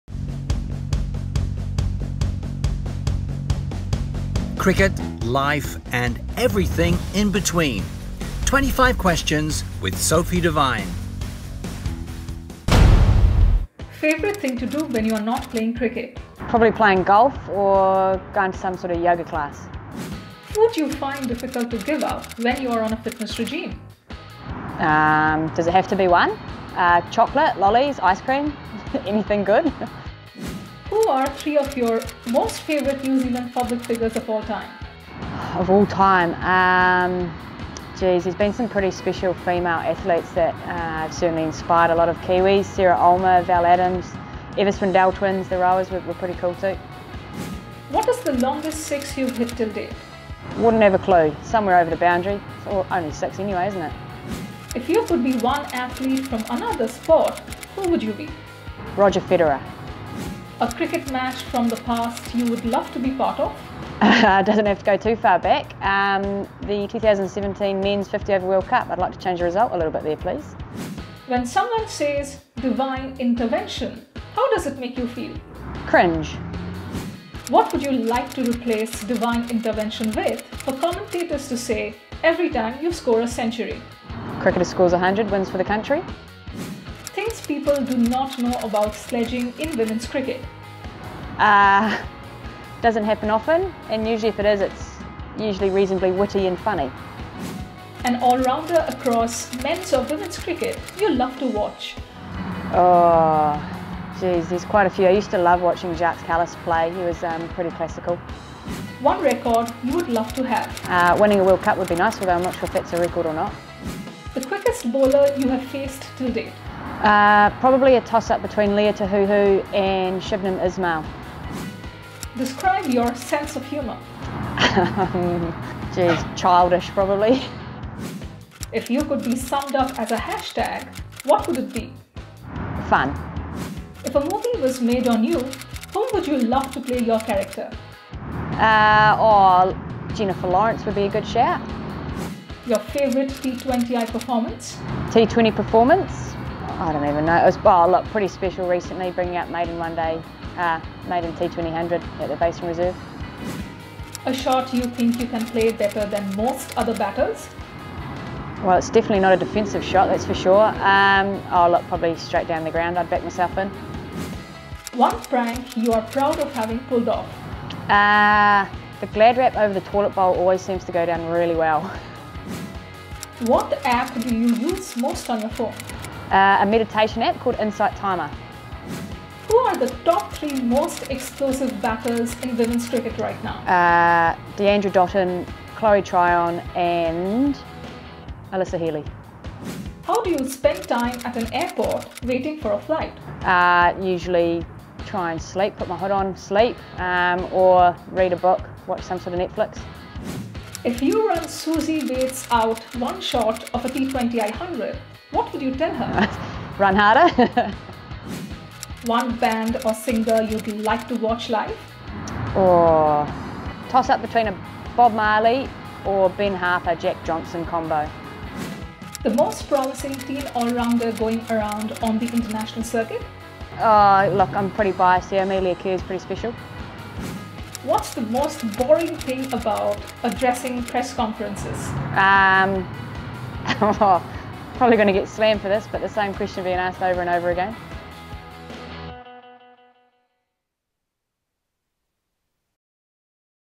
What cliché does Sophie Devine wish commentators would retire?
Hint: it's a groan-worthy pun on her name.